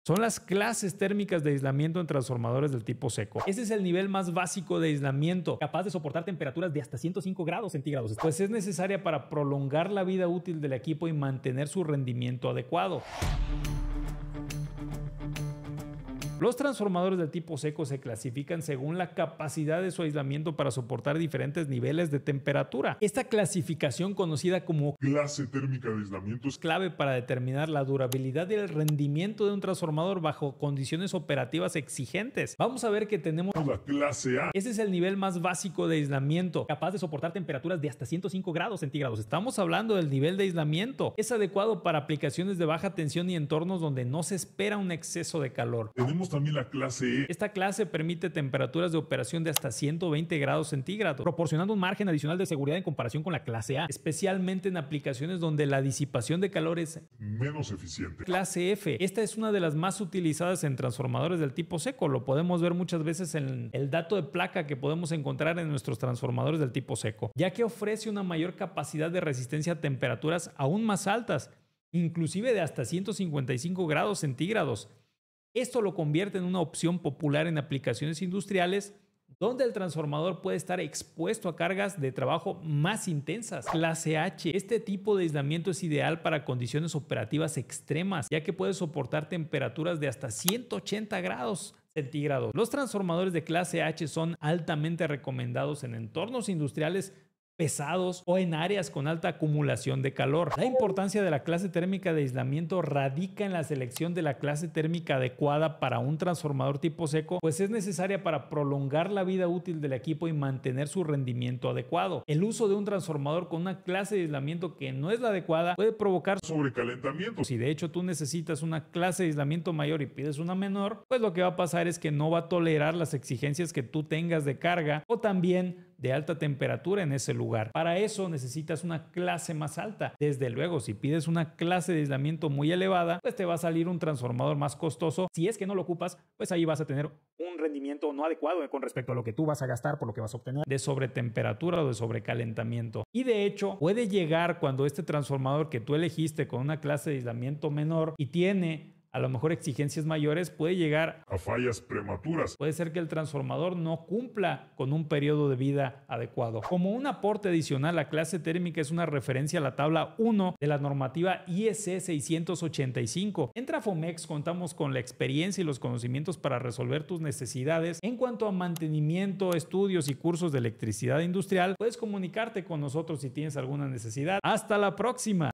AISLAMIENTO en TRANSFORMADORES SECOS
💊Recibe tu email diario sobre capacitación, consejos y enseñanzas en electricidad industrial

¿Sabías que las clases térmicas de aislamiento son clave para la durabilidad y seguridad de tus transformadores tipo seco?

En este video, te explicamos todo lo que necesitas saber sobre las diferentes clases térmicas de aislamiento y cómo elegir la adecuada puede proteger tu inversión y evitar costosos fallos.

Descubre cómo el aislamiento térmico adecuado prolonga la vida útil de tus transformadores, mejora su rendimiento y garantiza la seguridad de tus instalaciones eléctricas.

 ¡No te pierdas esta información esencial para cualquier profesional en electricidad industrial!